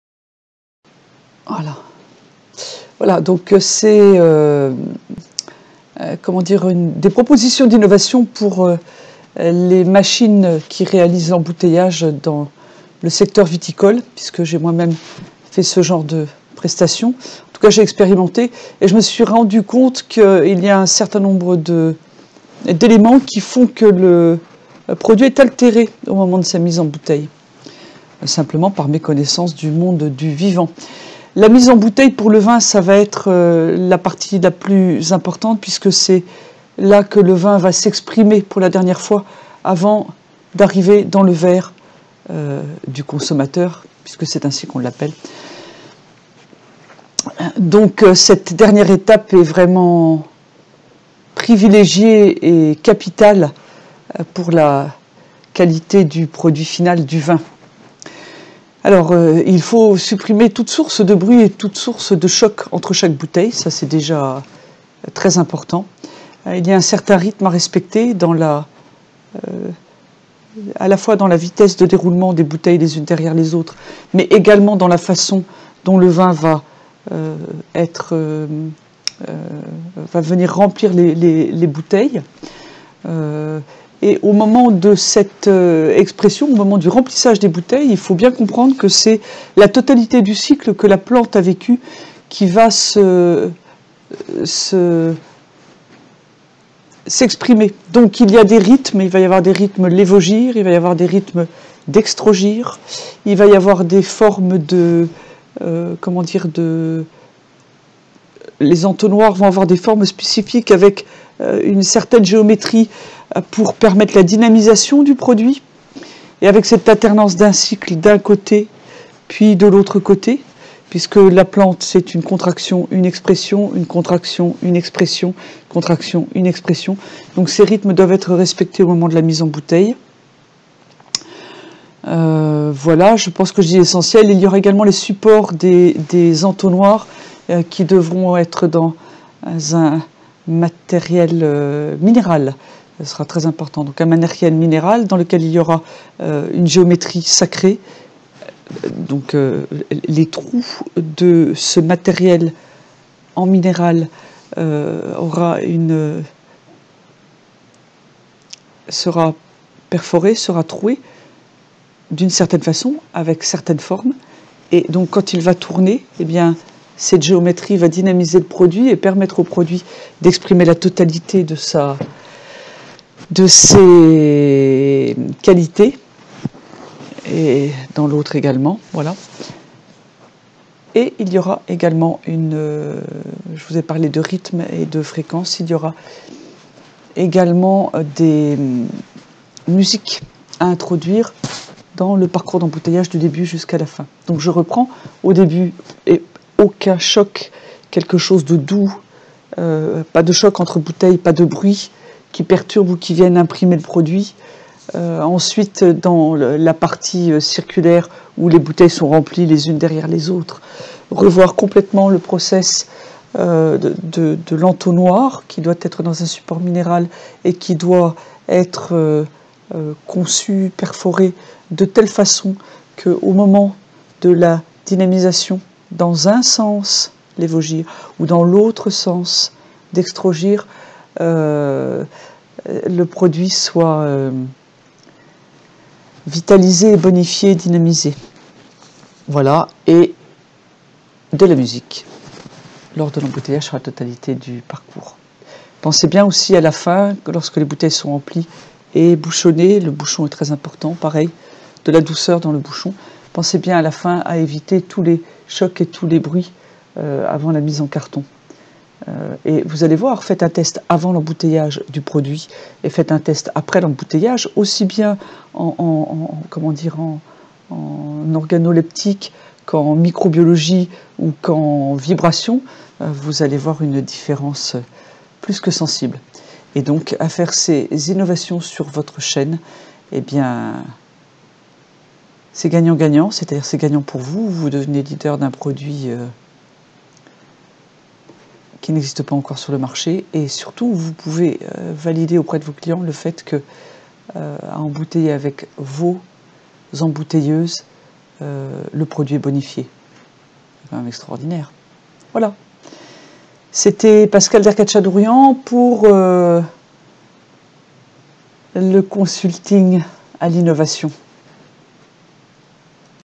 Vivant et embouteillage
Le vivant c'est aussi dans l 'Industrie, avec les chaînes d'embouteilleuses qui mériteraient d'innover! Des propositions simples!